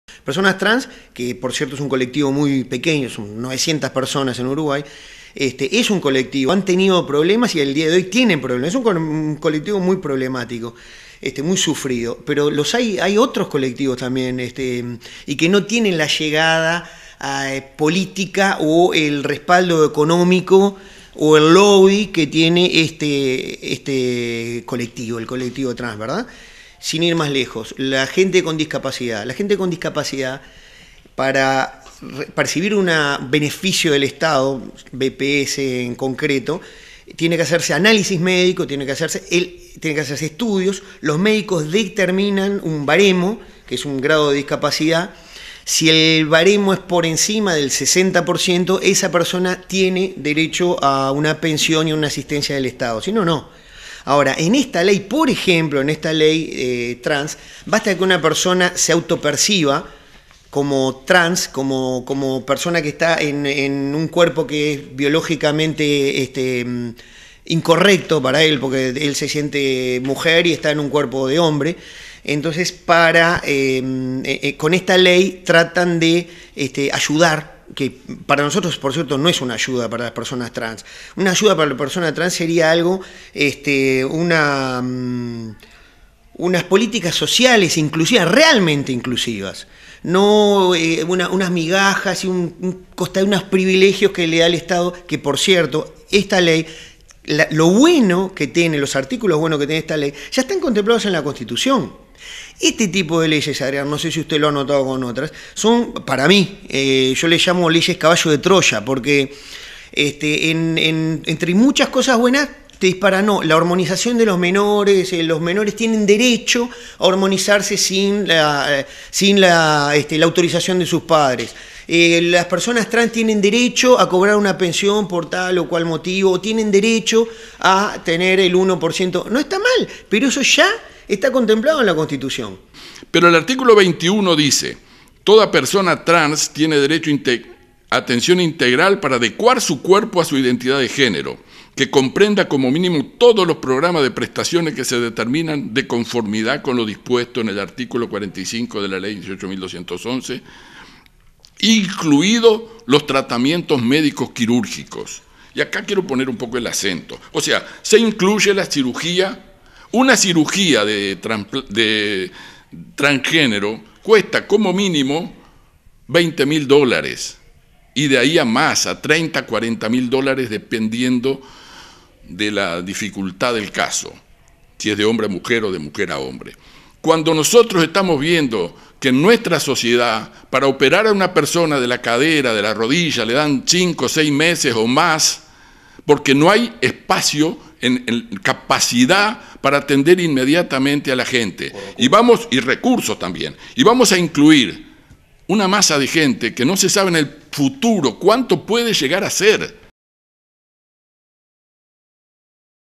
Un grupo de ciudadanos salteños marcaron sus diferencias con la Ley Trans.
Sobre este tema un grupo de ciudadanos salteños marcaron sus diferencias con la Ley Trans. Para estas personas, el colectivo trans recibe un importante beneficio con la ley, caso que no pasa con otras personas que padeces de diferentes discapacidades. Se dijo además que la ley le otorga a la personas trans la posibilidad de operarse de forma gratuita, teniendo estas intervenciones un costo aproximado de unos 20 mil dólares, mientras que un ciudadano uruguayo debe de esperar hasta 5 meses para intervenirse porque no existe capacidad en el sistema de salud para abordar a tiempo todos los casos.